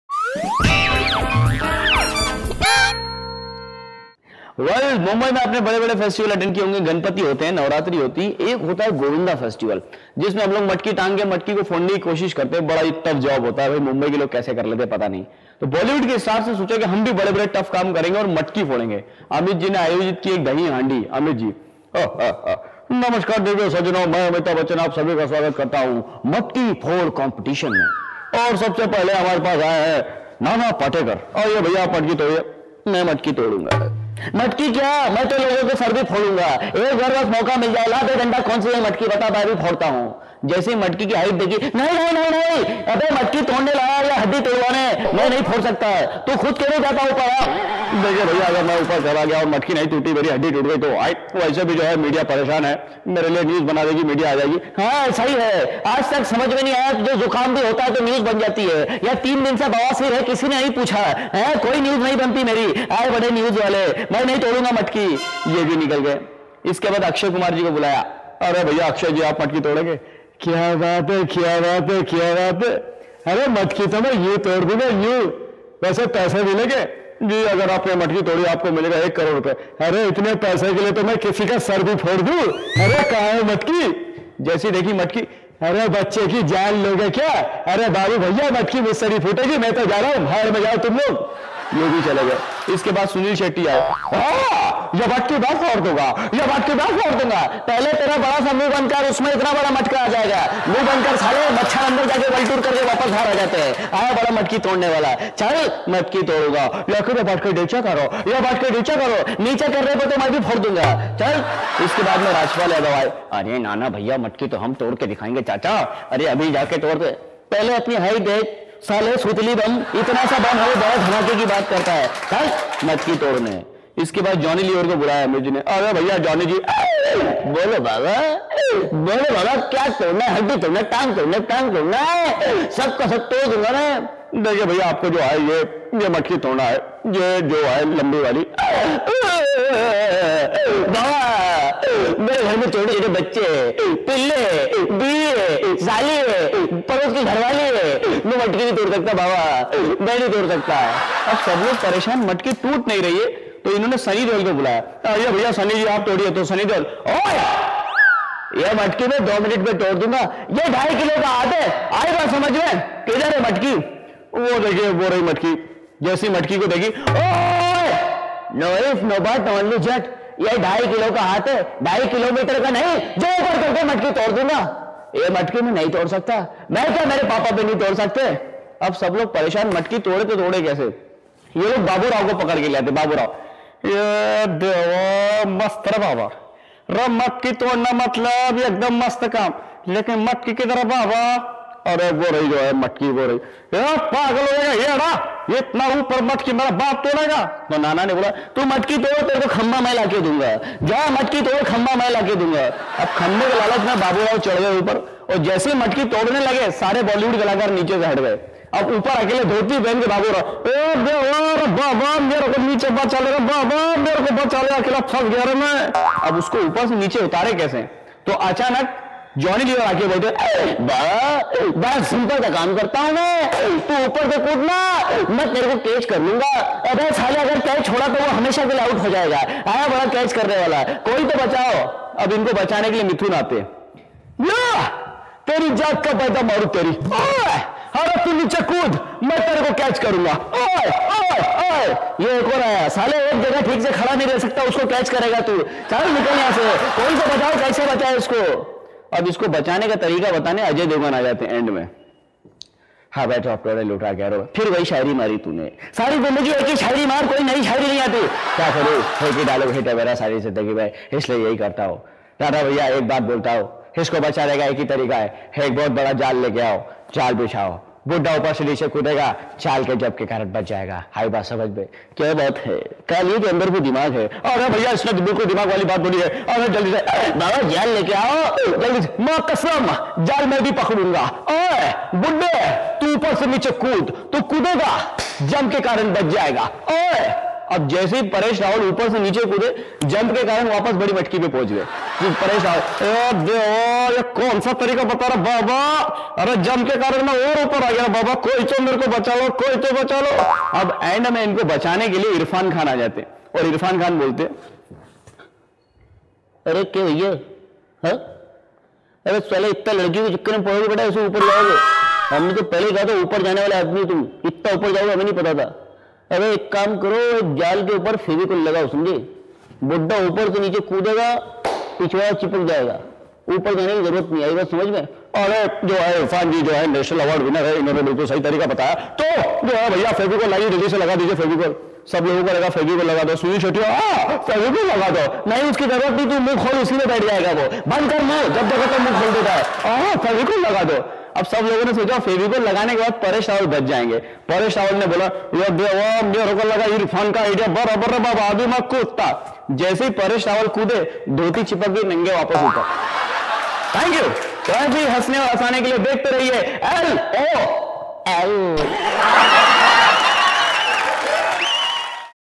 Mimicry Of Bollywood Actors LOL
Best video of mimicking some of the bollywood celebrities on celebrating the festival of Govinda.(janmashtami)